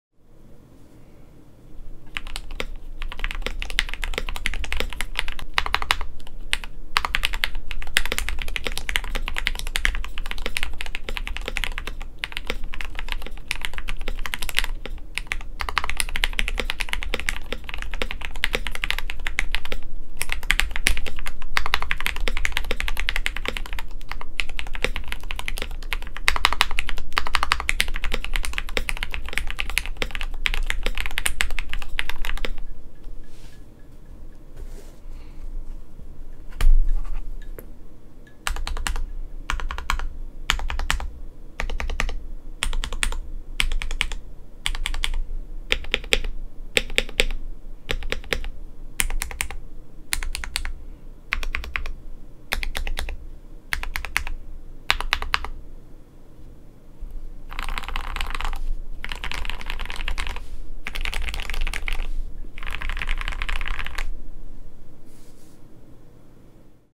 KBD67lite w/ Lubed Gateron Yellow Switches Typing Test ASMR
Keyboard: KBD67lite R1
Plate Material: Polycarbonate
Stabilizers: Lubed V2 Cherry Screw-Ins
Keycaps: Cherry Childhood (PBT)
Switches: Lubed Gateron Yellows (KS-3 Variant)
Films: N/A
Lube: Krytox 205g0
Foam: Stock Foam

Keyboard for a friend. Gat yellows are underrated ngl